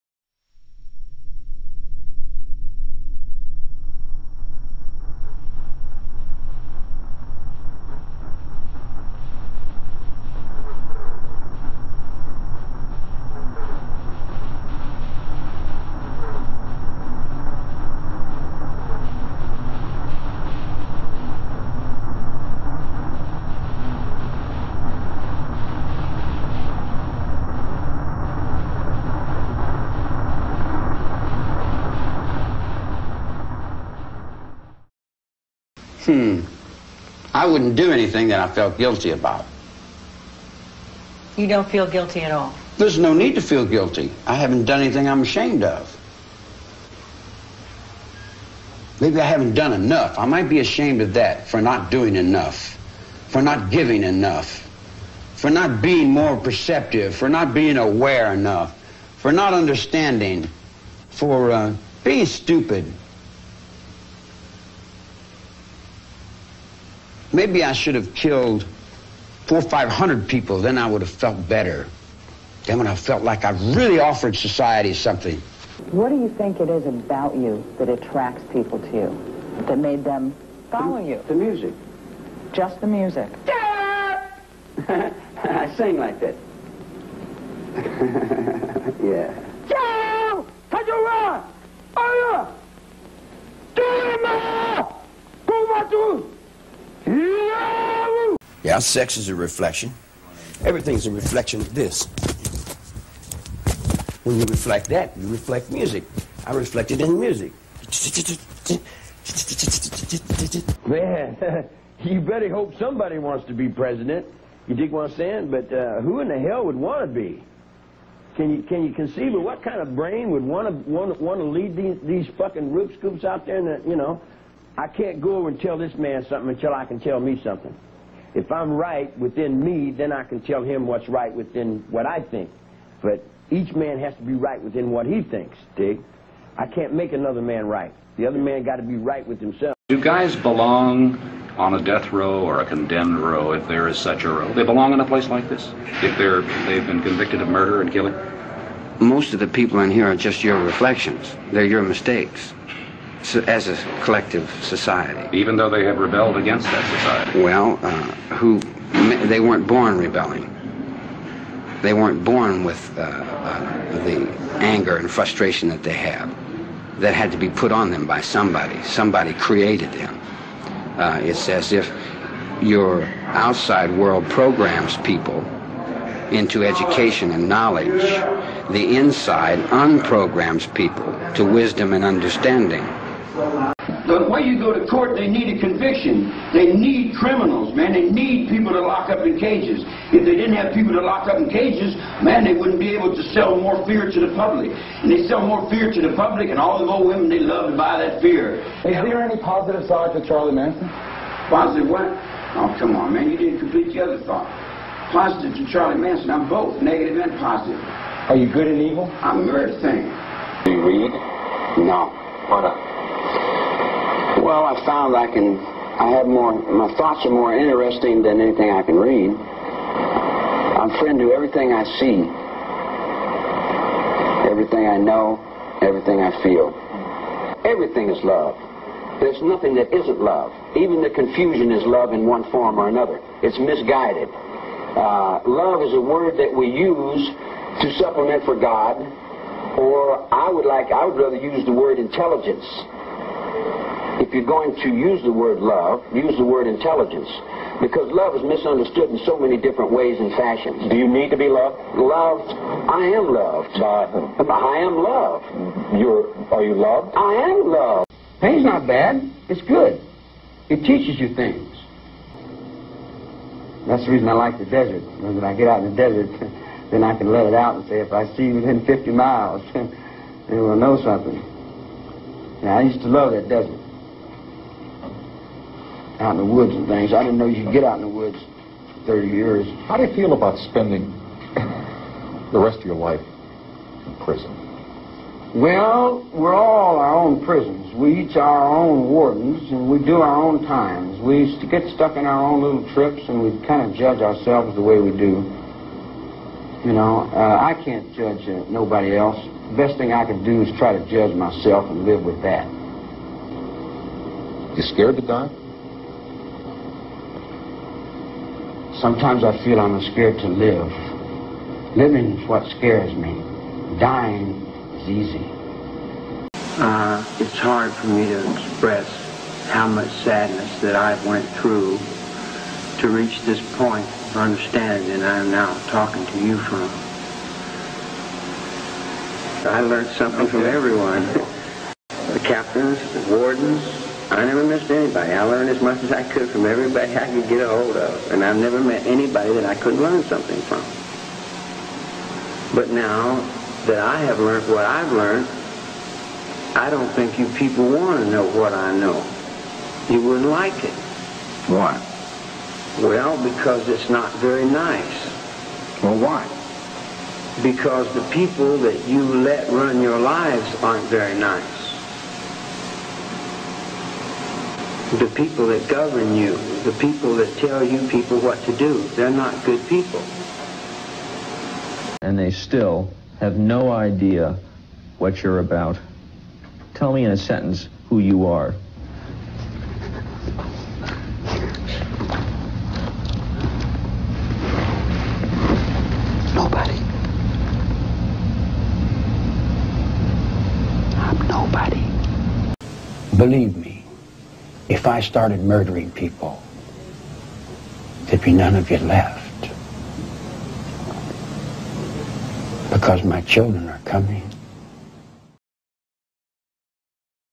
The Charles Manson Perception
These are a collection of interviews from multiple county jails/prisons in California recorded by news reporters and lawyers. The craziest thing was he never actually killed anyone personally, he had people do it for him. But that doesn't mean his intentions were pure. Just another very interesting individual in American history.